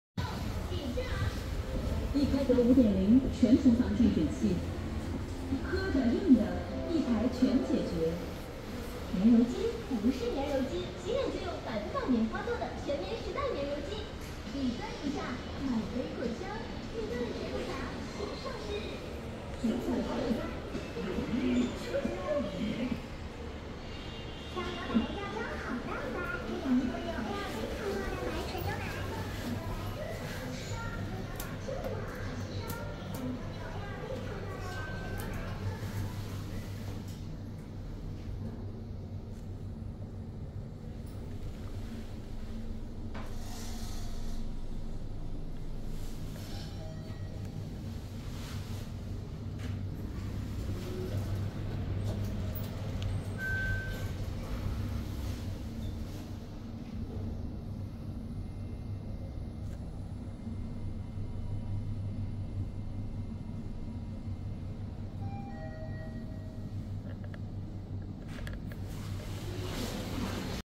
深圳羅湖地王 · 星薈三菱升降機
地點: 廣東省深圳市羅湖區深南東路5002號地王 · 星薈
載重: 1500公斤
乘人: 20人
投入服務年份:1996
途經樓層: B2, B1, LG, G, 2-4

Location: LANDMARK, 5002 Shennan East Road, Luohu, Shenzhen, Guangdong Province
Maximum Load: 1500kg
Maximum Capacity: 20person(s)
Year of commission: 1996
Floors passing through: B2, B1, LG, G, 2-4